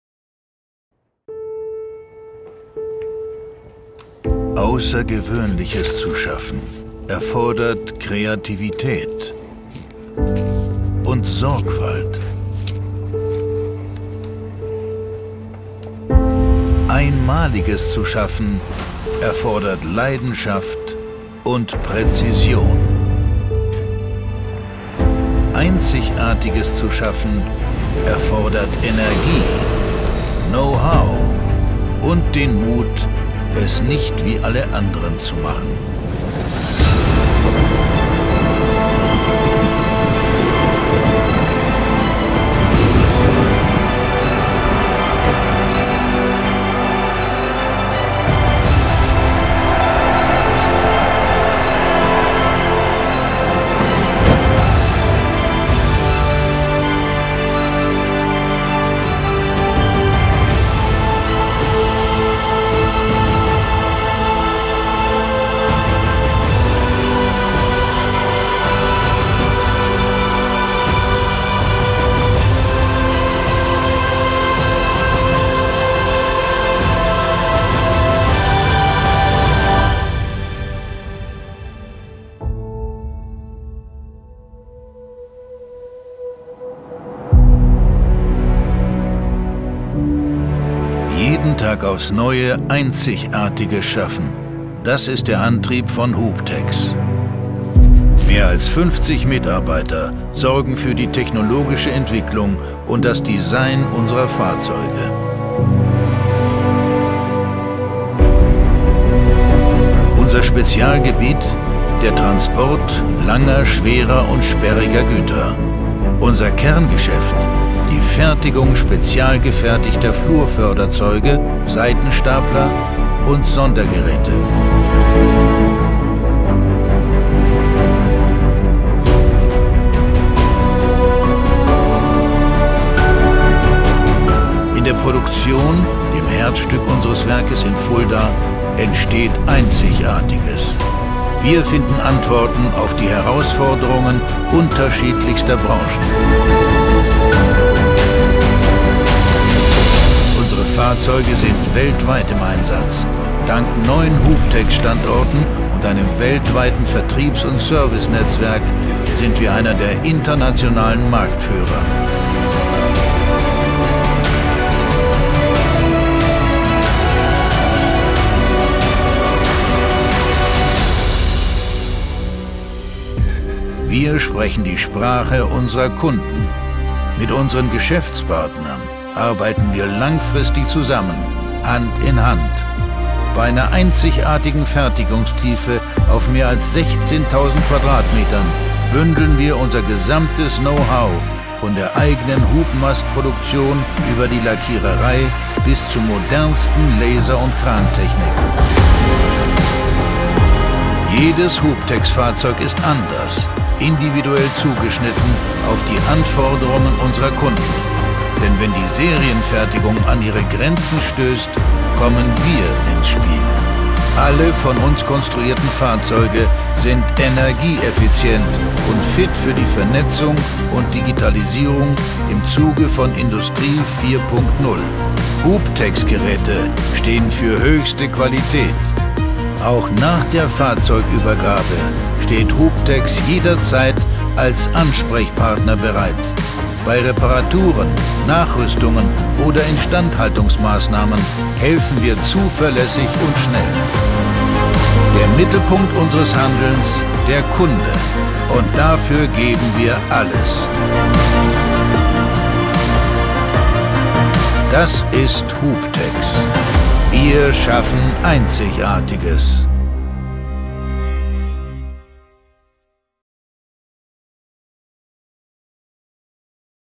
Imagefilm Hubtex Maschinenbau -Sondermaschinen, Stapler, Luftfahrt, Glas, Holzbau - Transport
Eine Produktion eines Imagefilms im Bereich Sondermaschinenbau ist keine einfache Sache.
Für unseren Kunden der Hubtex GmbH & Co. KG die Stapler, Seitenstapler, Mehrwegestapler produziert, sind wir dieses Projekt angegangen. Die Hubtex baut Geräte für die Bereiche
Schubmaststapler, Glastransport, Luftfahrt, Air Cargo, Gießerei, Holzbau,

Produkte vorallem Sondermaschinen sind mit ihrem Anwendungsbereich eng verknüpft. Diesen Fakt haben wir aufgegriffen und waren deshalb auch bei den Kunden vor Ort. Drehs im Winter, im Frühjahr, im Sommer. An kalten, warmen & nassen Tagen.
Die Drehorte verteilten sich auf ganz Europa ( Schweiz - St. Moritz, Frankfurt, Dortmund, Mannheim, München, Hamburg, Stuttgart, uvm.

Natürlich wurde auch vor Ort im Werk der Hubtex gedreht.

Equipment:
Audio Neumann 185 | Audio Sennheiser MKH 416 | Kamera Red Scarlet W | Licht Sunbounce Pro | Astra Litepanels 1x1 | Stativ Saechtler | DJI RONIN und Freefly Movi | Zeiss | Litepanels 1x1 | Astera Titan Tube | FreeflyMovi M10